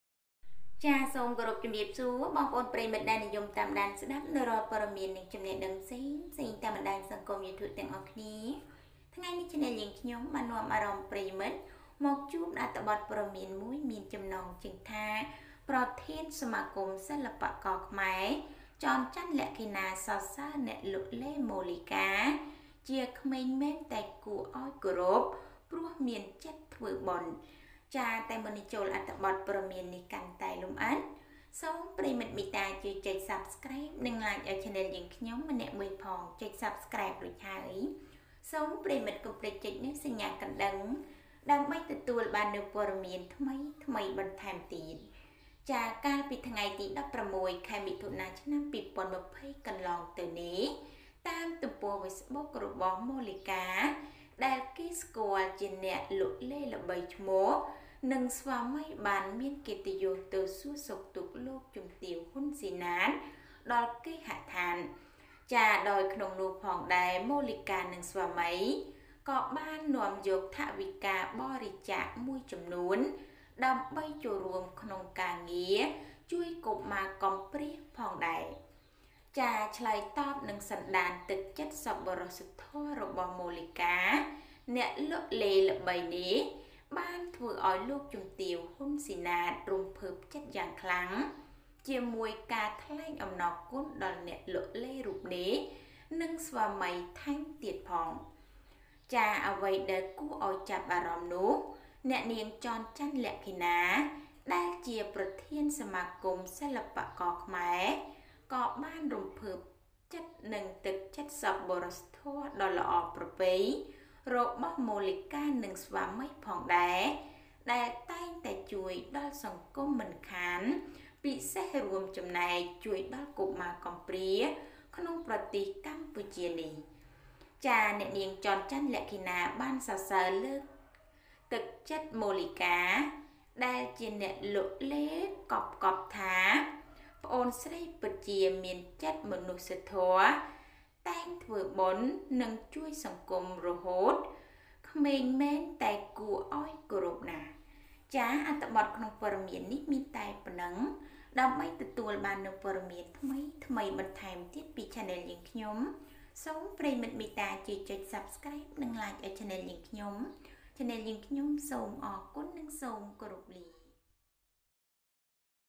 ប្រធានសមាគមសិល្បករខ្មែរ ចន ច័ន្ទលក្ខិណា សរសើរអ្នកលក់ឡេ ម៉ូលីកា «ជាក្មេងមែនតែគួរឱ្យគោរពព្រោះមានចិត្តធ
ប្រធានសមាគមសិល្បករខ្មែរ ចន ច័ន្ទលក្ខិណា សរសើរអ្នកលក់ឡេ ម៉ូលីកា ជាក្មេងមែនតែគួរឱ្យគោរពព្រោះមានចិត្តធ្វើបុណ្យ  President of Khmer Artist Association.